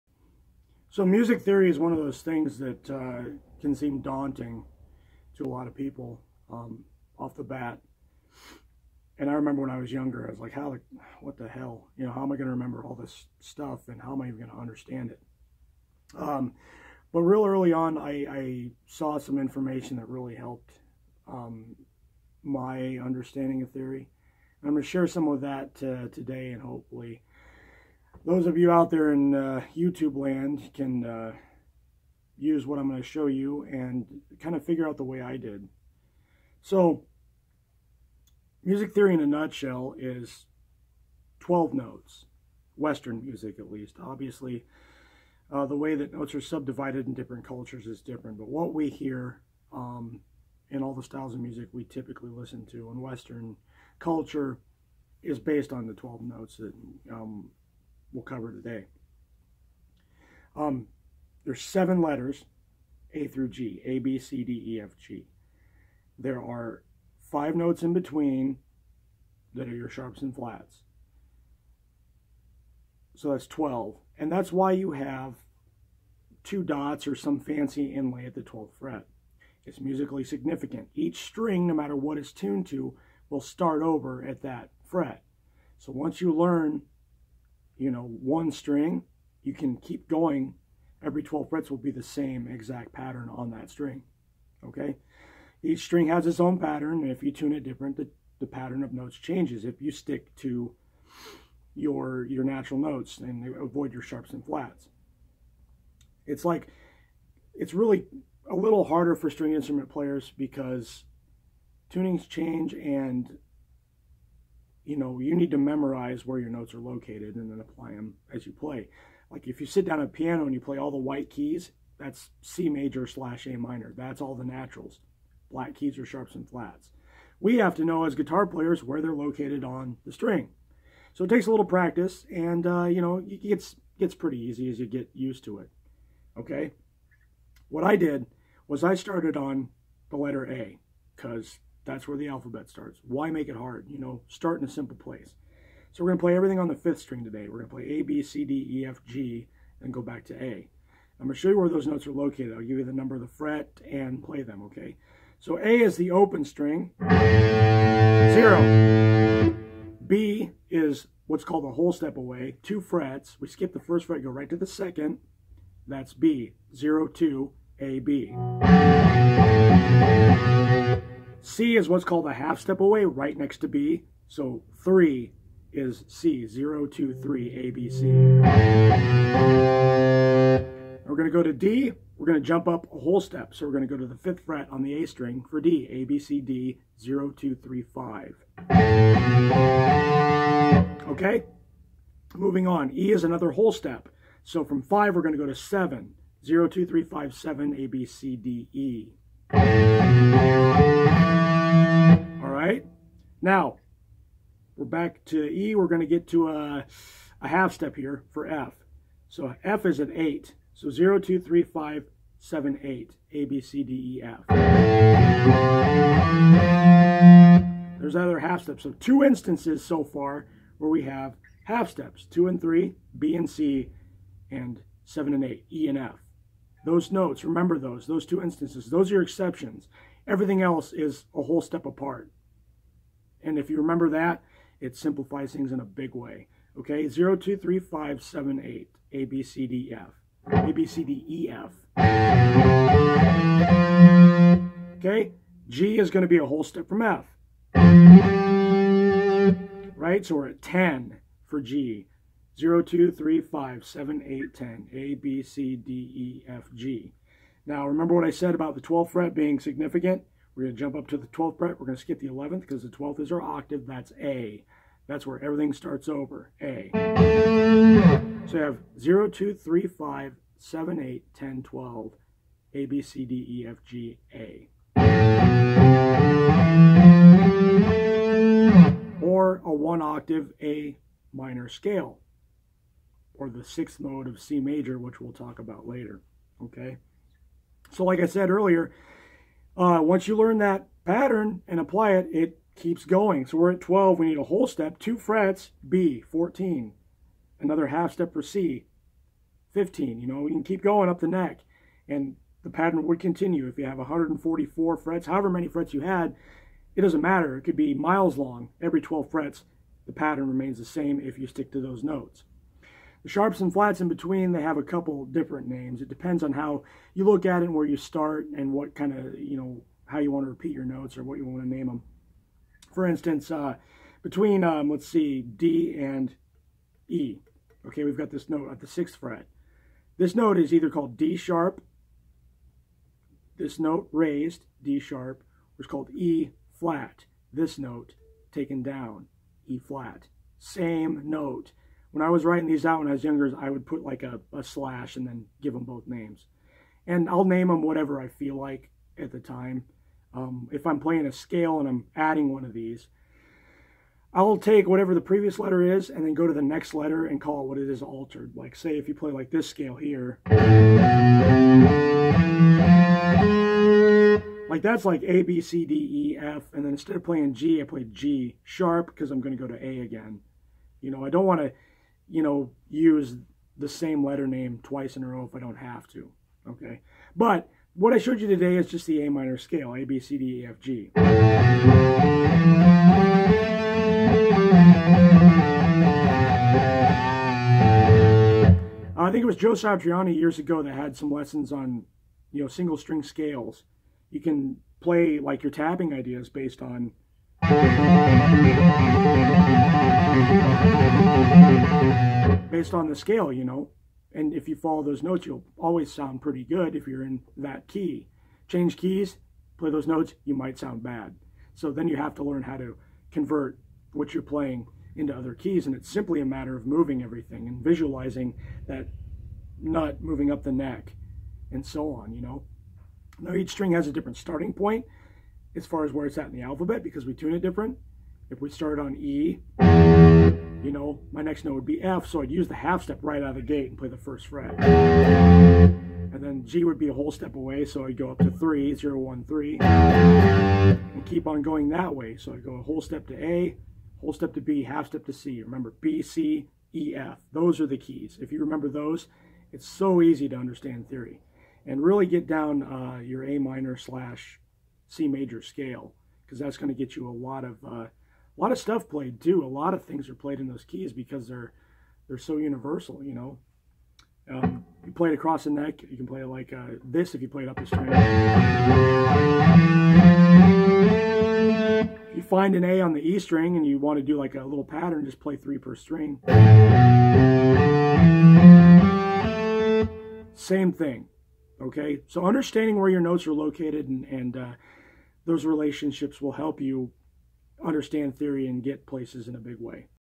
Basic Theory & Note Location
A quick overview of basic music theory and finding the notes on your guitar neck.